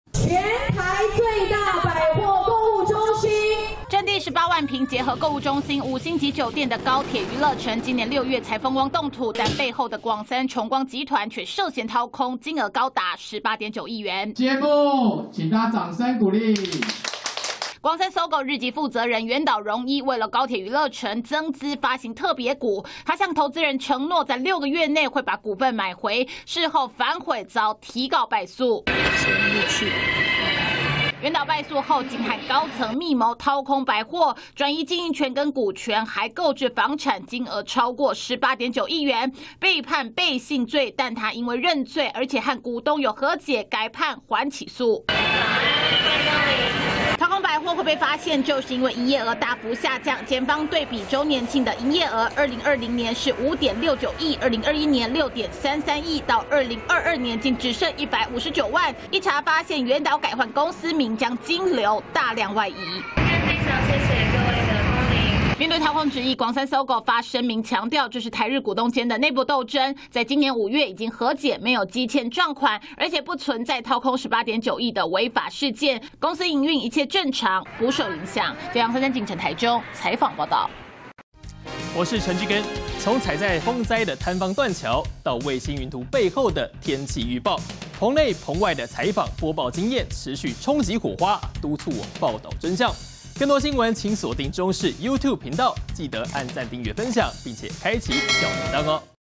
台中廣三SOGO爆掏空18.9億! 日籍負責人涉"背信罪"│中視新聞 20241028
台中廣三SOGO百貨，驚爆"案"!公司日籍負責人原島榮一，涉嫌掏空公司近19億元，因為當初他為投資"高鐵娛樂城"，發行"特別股"增資，卻反悔不收回，遭股東提告且敗訴，原島榮一竟開始，轉移百貨經營權、股權，購置房產、外移刷卡金流，前前後後掏空18.9億元。